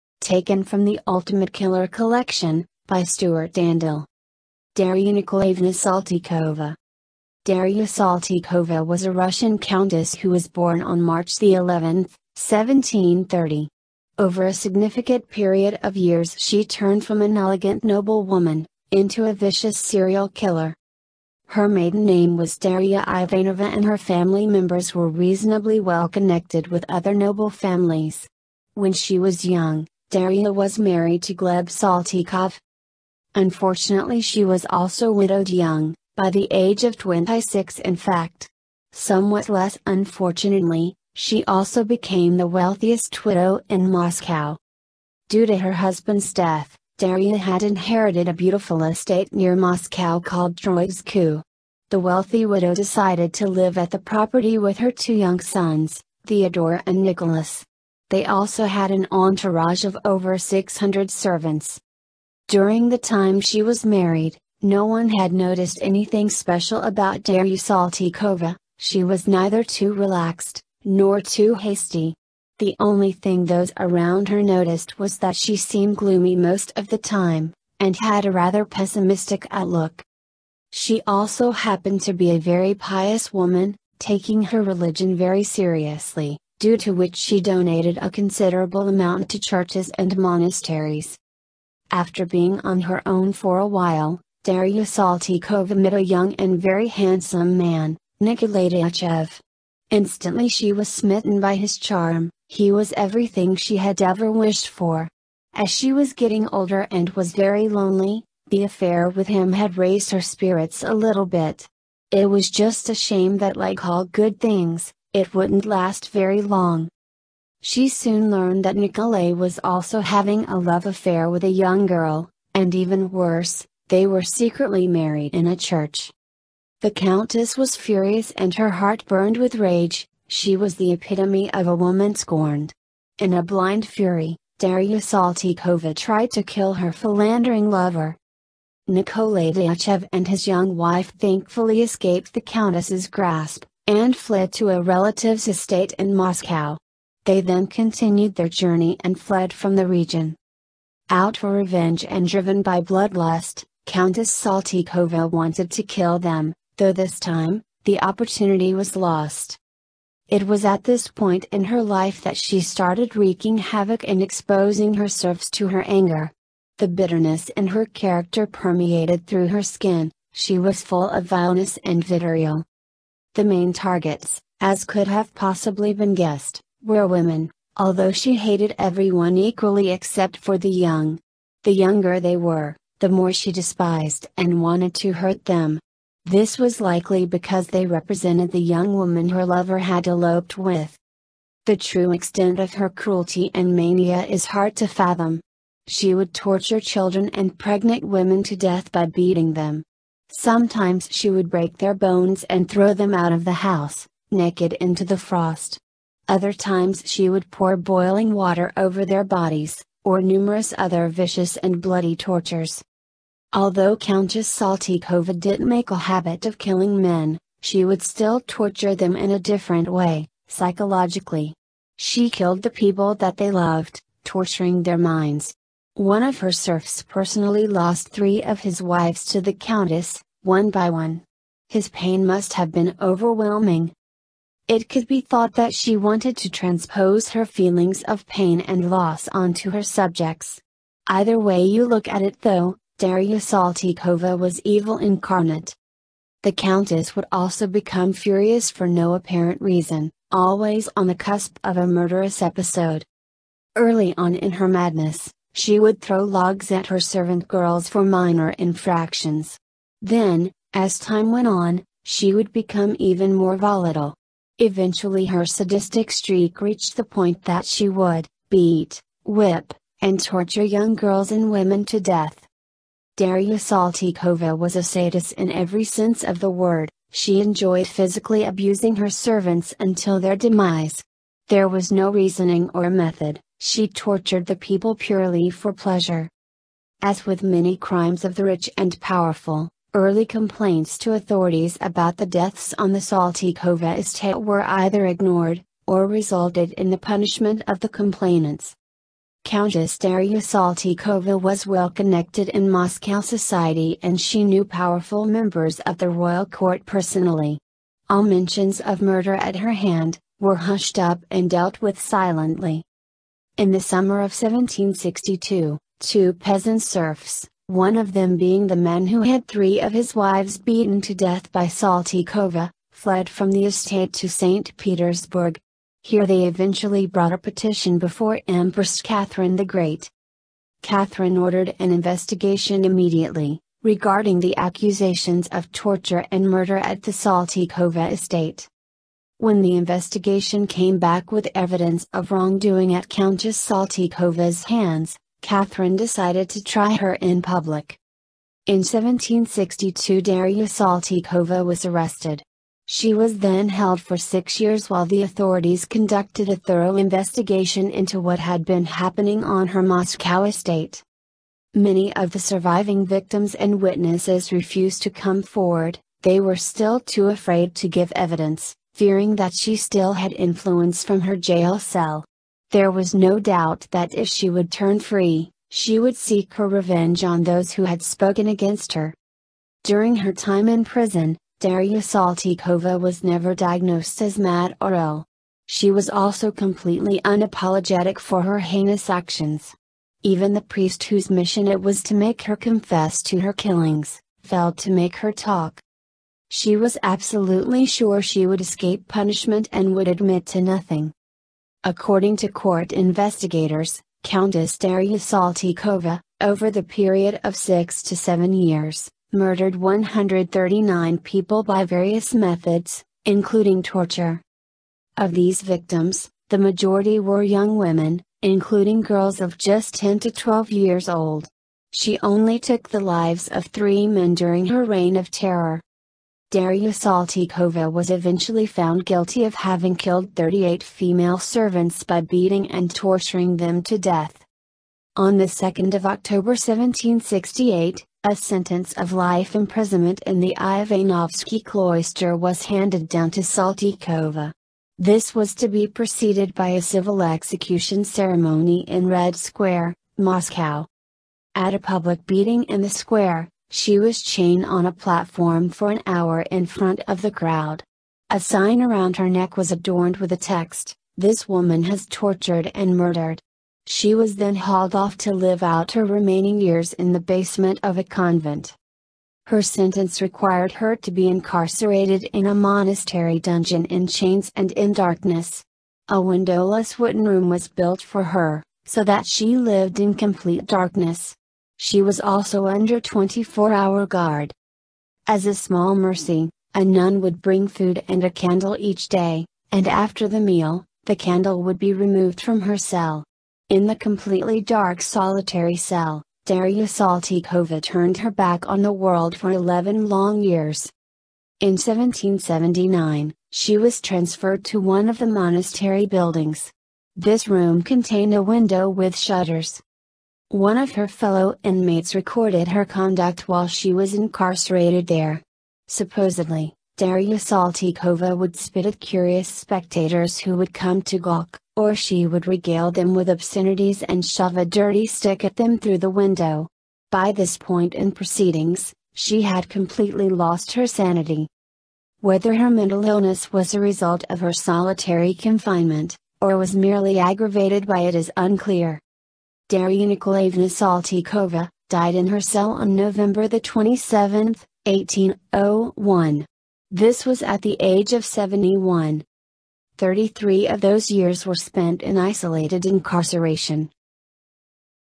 Darya Nikolayevna Saltykova - The Saltychika - Serial Killer
Biography of Darya Saltykova and her barbaric punishments for her servants. Taken from The Ultimate Killer Collection by Stewart Andel.